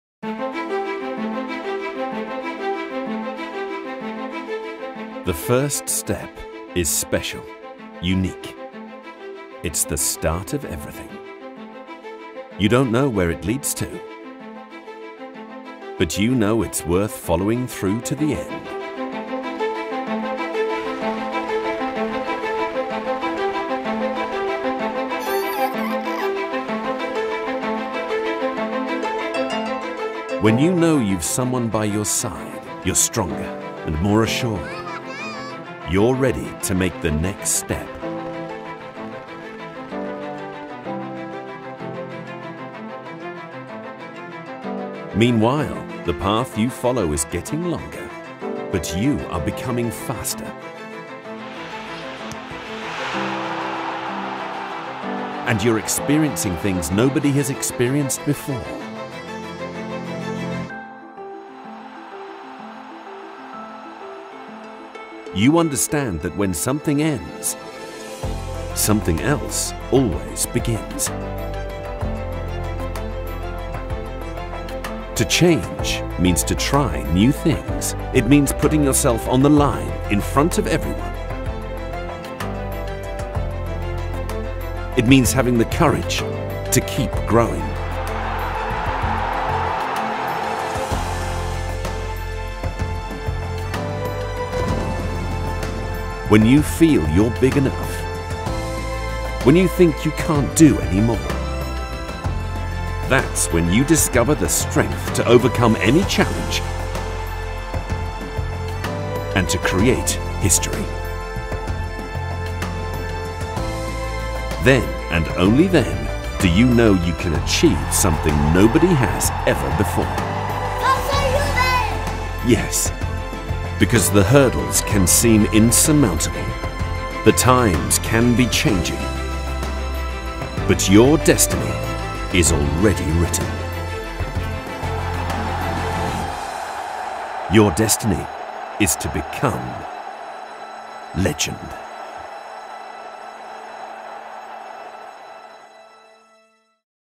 We Are LE6END
Six years later, we've reached our destiny. WE ARE LE6END.

Juventus win a sixth consecutive Scudetto title.